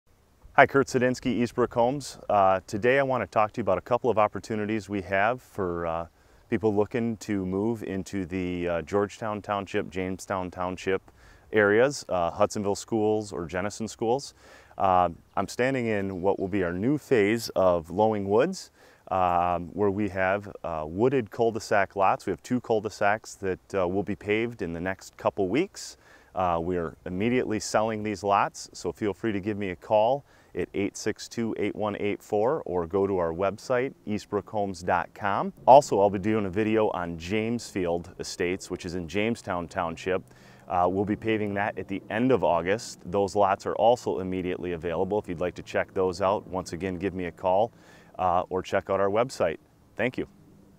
Lowing Woods - July 2016
Our newest Lowing Woods phase offers two wooded cul-de-sac's on the east side of the clubhouse! Great opportunity for a great new home in Hudsonville Schools! Contact Kurt Suidinski with Eastbrook Homes for more information!